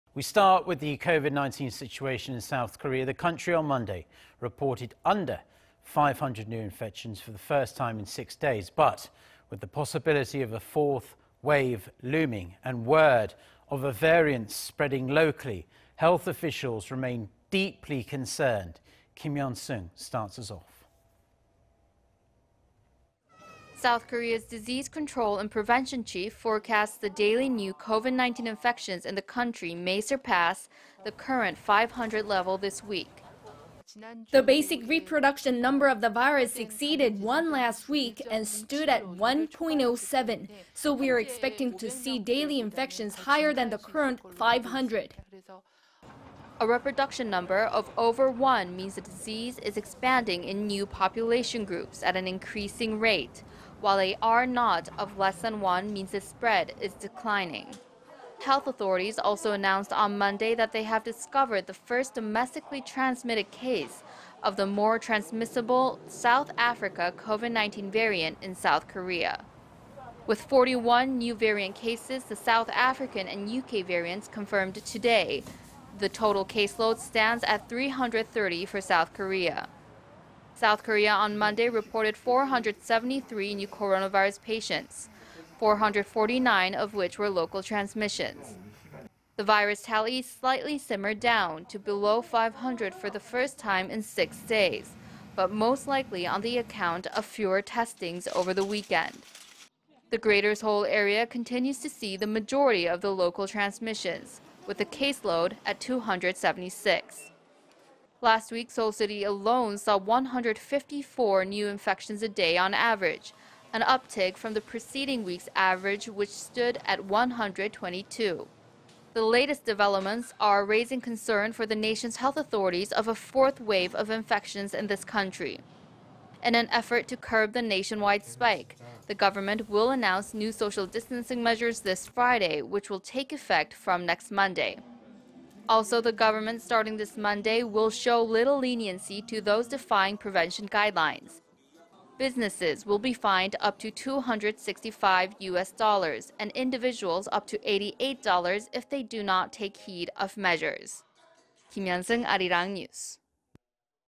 Health authorities grapple with worrisome fourth nationwide spike of COVID-19
4차 유행세의 가능성 성큼 앞으로 다가와... 남아공 변이 바이러스의 첫

We start with the COVID-19 situation in South Korea.
South Korea on Monday reported under 500 new infections for the first time in six days.
But with the possibility of a fourth wave looming, and word of a variant spreading locally, health officials remain deeply concerned.
Kim Yeon-seung reports. South Korea's disease control and prevention chief forecasts the daily new Covid-19 infections in the country may surpass the current 500 level this week.

"The basic reproduction number of the virus exceeded 1 last week, and stood at 1-point-0-7. So we're expecting to see daily infections higher than the current 500."

A reproduction number of over one means the disease is expanding in new population groups at an increasing rate while a R-naught of less than one means the spread is declining.

Health authorities also announced on Monday that they have discovered the first domestically transmitted case of the more transmissible South Africa Covid-19 variant in South Korea.
With 41 new variant cases - the South African and UK variants - confirmed today, the total caseload stands at 330 for South Korea.

South Korea on Monday reported 473 new coronavirus patients,... 449 of which were local transmissions.
The virus tally slightly simmered down... to below 500 for the first time in 6 days,... but most likely on the account of fewer testings over the weekend.
The greater Seoul area continues to see the majority of the local transmissions... with the caseload at 276.
Last week, Seoul city alone saw about 154 new infections a day on average.
An uptick from to the preceding week's average which stood at 122.

The latest developments are raising concerns for the nation's health authorities of a fourth wave of infections in this country.

In an effort to curb the nationwide spike, the government will announce new social distancing measures this Friday, which will take effect... from next Monday.
Also, the government starting this Monday,... will show little leniency to those defying prevention guidelines.
Businesses will be fined up to 265 U.S. dollars, and individuals up to 88 dollars if they do not take heed of measures.
Kim Yeon-seung, Arirang News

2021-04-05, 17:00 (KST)